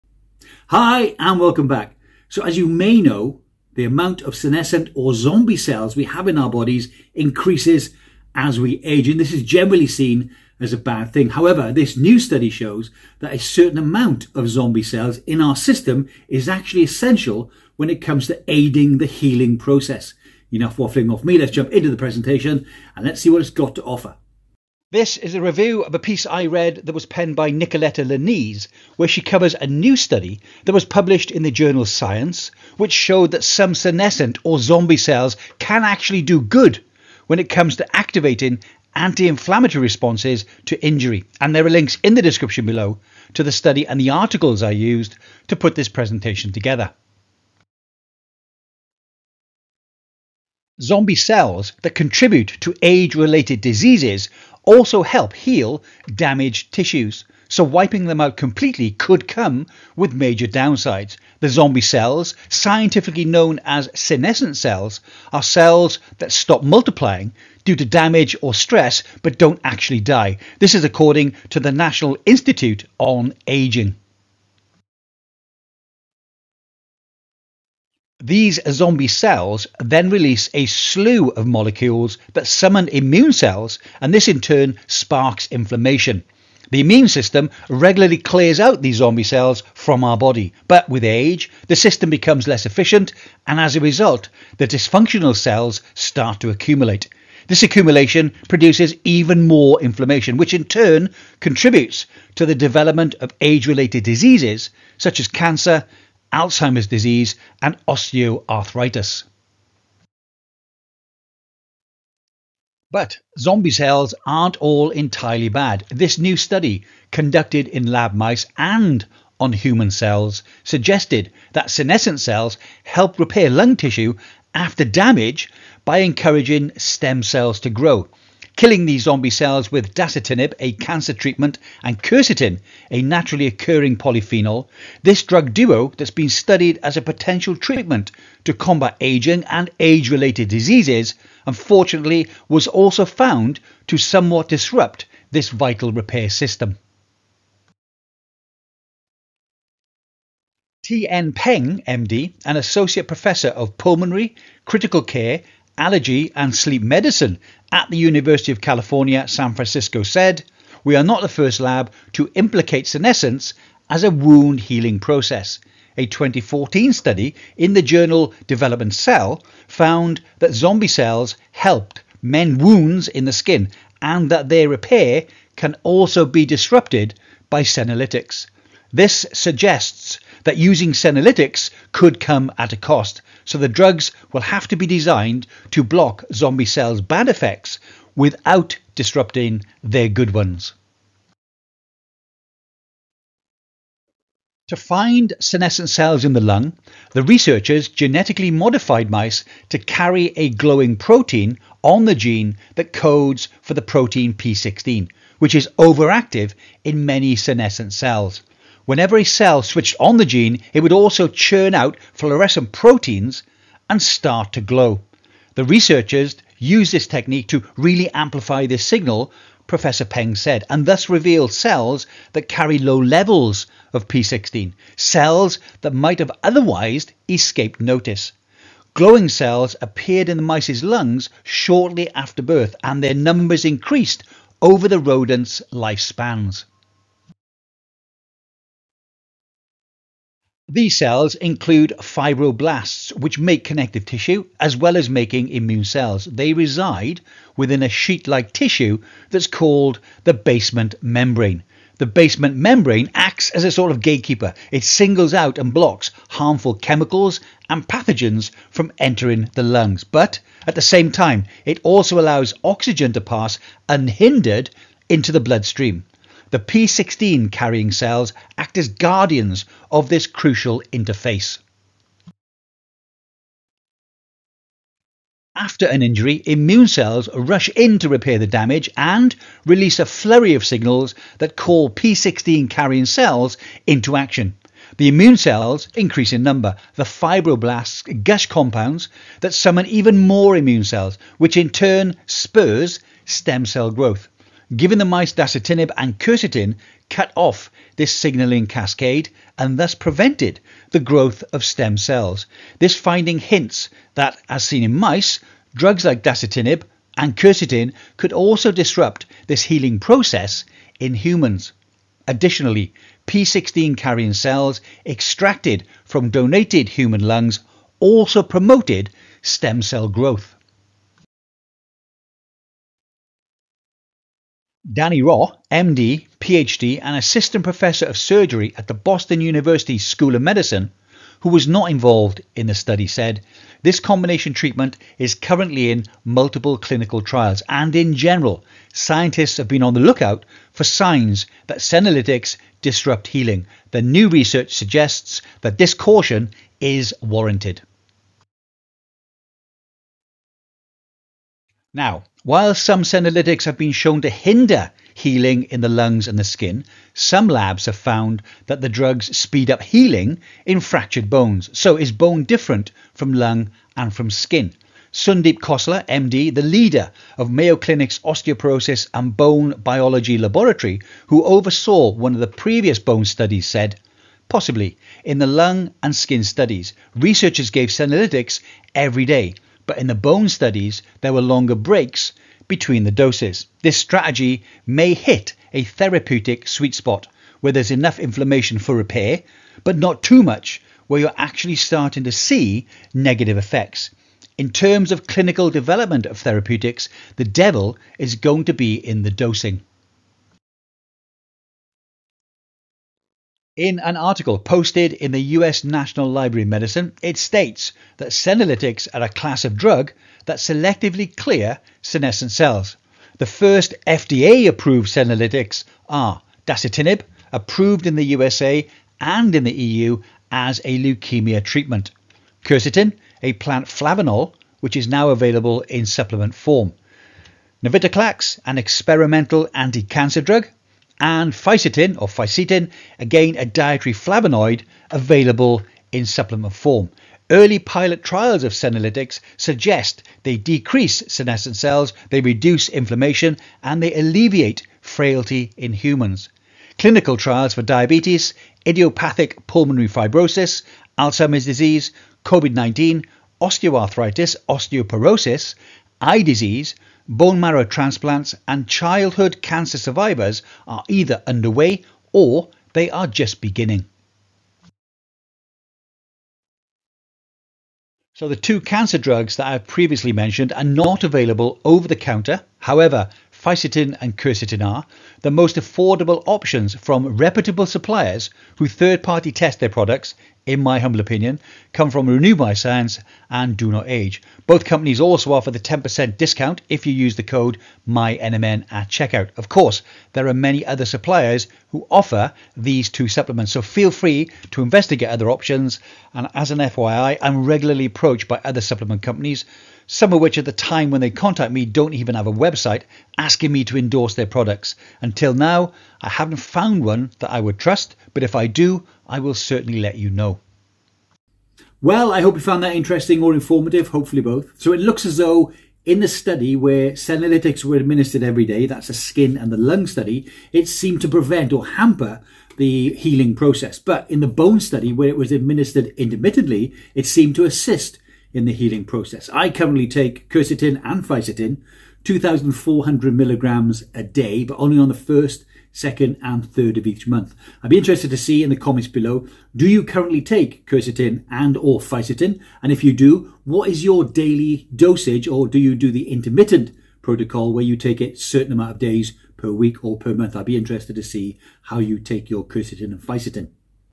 New Research: Not ALL Senescent Cells are Bad!
As we age the number of senolytic cells or zombie cells we have in our body increases, this is a bad thing, although a new study seems to show that some zombie cells actually help in the healing process.

• 2,400mg of Fisetin, on the 1st, 2nd & 3rd of each month
• 2,400mg of Quercetin, on the 1st, 2nd & 3rd of each month
• 800mg SIRT6 Activator
• 600mg DIM

"Zombie cells" that contribute to age-related diseases also help heal damaged tissues, so wiping them out could come with major downsides. The zombie cells, scientifically known as "senescent" cells, are cells that stop multiplying due to damage or stress but don't die; this according to the National Institute on Aging. These zombie cells then release a slew of molecules that summon immune cells and this in turn spark inflammation.
The immune system regularly clears these zombies from the body, but with age, the system becomes less efficient; and as a result, the dysfunctional cells accumulate. This accumulation produces more inflammation, which in turn contributes to the development of age-related diseases such as cancer, Alzheimer's disease and osteoarthritis.
But zombie cells aren't entirely bad. This new study, conducted in lab mice and on human cells, suggested that senescent cells helped repair lung tissue after damage by encouraging stem cells to grow. Killing these zombie cells with Dasatinib, a cancer treatment, and Quercetin, a naturally occurring polyphenol – a drug duo that's been studied as a potential treatment to combat aging and age-related diseases - was found to somewhat disrupted this repair.
Tien Peng MD, an associate professor of pulmonary, critical care, allergy and sleep medicine at the University of California, San Francisco said
"We are not the first lab to implicate senescence as a wound-healing process. A 2014 study in the journal Developmental Cell found that zombie cells helped mend wounds in the skin and that their repair can also be disrupted by senolytics. This suggests that using senolytics could come at a cost, so the drugs will have to be designed to block zombie cells' bad effects without disrupting their good ones.”
To find senescent cells in the lung, researchers genetically modified mice to carry a glowing protein on the gene that codes for the protein "P16," which is overactive in many senescent cells. Whenever a cell switched on the gene, it would also churn out fluorescent proteins and start to glow. The researchers used this technique to "really amplify this signal," Professor Peng said, and thus revealed cells that carry low levels of p16; cells that may have otherwise escaped notice. Glowing cells appeared in mice's lungs shortly after birth, and their numbers increased over the rodents' life spans. The cells included fibro-blasts, which make connective tissue, as well as immune cells; they reside within a sheet-like tissue called the "basement membrane." The basement membrane is a sort of gatekeeper, it singles out and blocks harmful chemicals and pathogens from entering the lungs. But, at the same time it also allows oxygen to pass unhindered into the bloodstream.